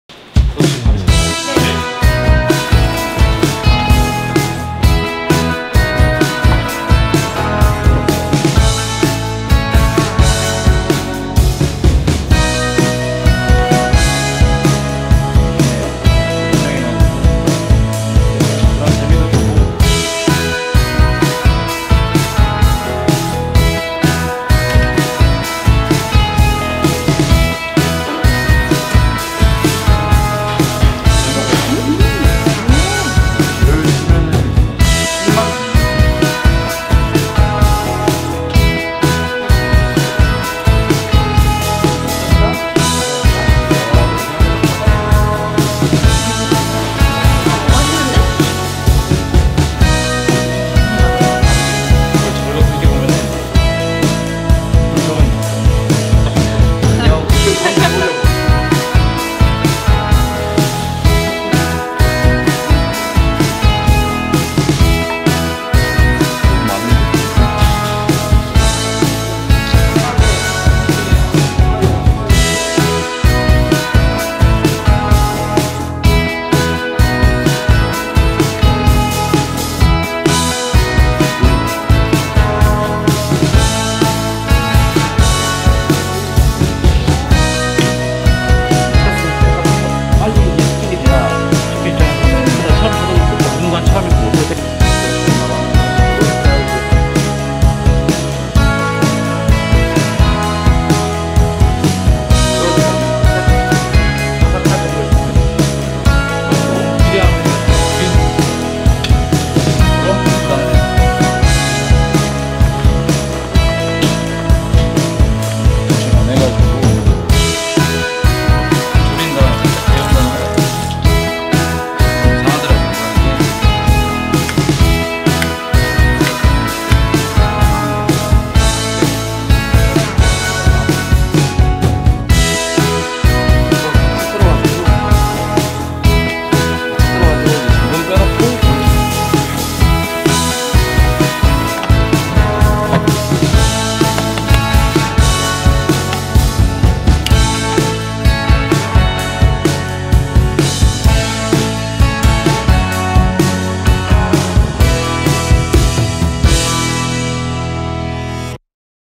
[2022한식브랜드클래스] 한식특강_박주은셰프
2022년 한식진흥원과 농림수산식품부 후원으로 청강대 푸드스쿨에서 진행하는 한식브랜드클래스 수업. 현대적인 한식을 추구하는 레스토랑 주은의 박주은 총괄셰프님께서 직접 학교로 오셔서 자신만의 한식에 대한 철학과 대표 요리를 선보여 주었습니다.
금태조림과 농어물회, 그리고 밤강정이었는데요. 정성스런 손길과 셰프님만의 노하우를 전수받는 시간이었습니다.